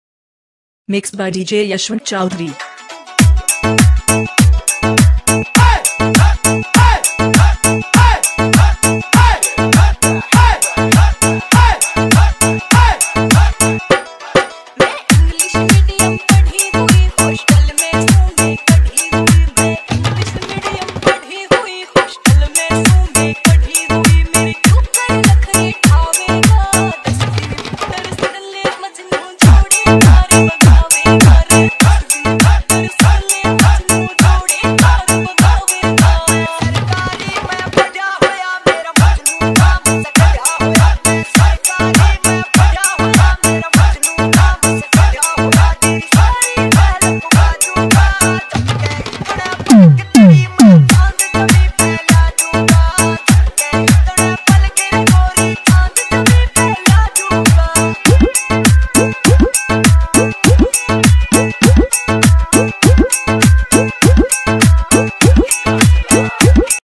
VICKY KAJLA 2018|| GRAND OPENING LIQUID BEER CAFE
आज की शाम दिल्ली वालों के नाम

METRO WALK, ADVENTURE ISLAND ROHINI

GRAND OPENING